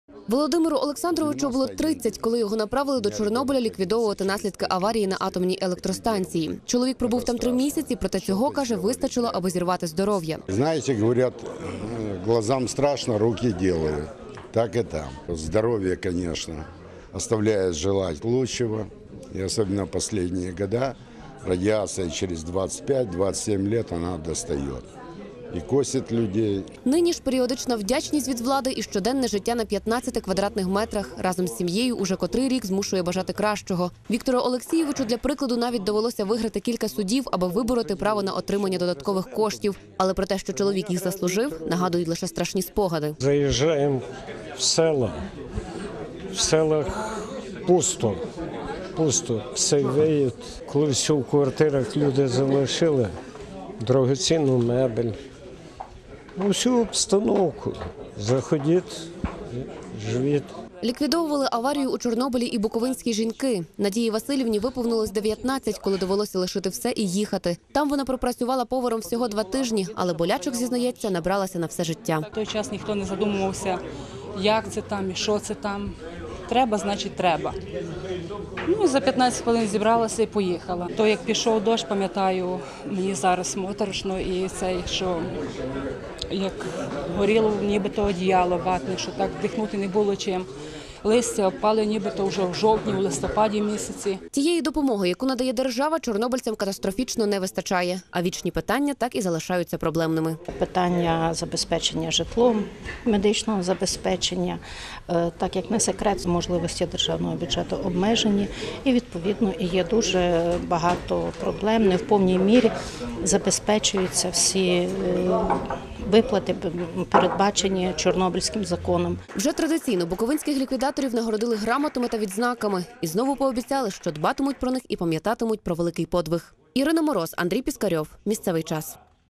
Вшанування чорнобильців
У Чернівцях вшанували учасників ліквідації наслідків аварії на Чорнобильській АЕС. Нагадаємо, 14 грудня 1986 року було завершено будівництво саркофагу над зруйнованим четвертим енергоблоком атомної електростанції. Щороку цього дня відзначається — День ліквідатора. На Буковині проживає понад 20 тисяч людей, яких торкнулась чорнобильська біда.